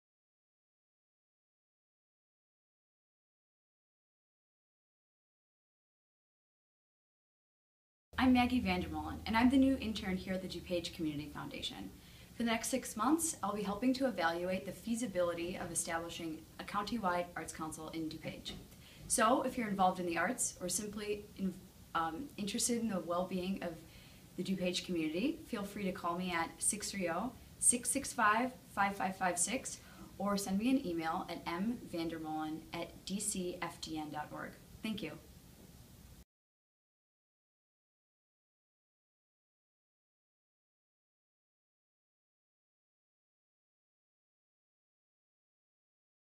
Maggie VanDerMolen
Maggie VanDerMolen has come on board as our new intern! She will be working closely with Barb Szczepaniak, Director of Programs, and Cheryl Yuen, nationally renowned art consultant, for the duration of the All for Arts study to establish the feasibility of a county-wide arts council in DuPage.
Maggie grew up in Oak Park, IL and lived there until she graduated high school. After a year of studying theology and dance in Switzerland, she moved to Northfield, MN to where she attended St. Olaf College. She graduated cum laude in May 2014 with a Bachelors of Arts in Art, Activism, and Social Change and a Concentration in German Studies. Her previous work in DuPage county was for Exodus World Service, where she assisted in welcoming refugees to the Chicagoland area. She has also worked for a puppet and mask theatre in Minneapolis and for non-profit committing to rehabilitating prisoners in South Africa.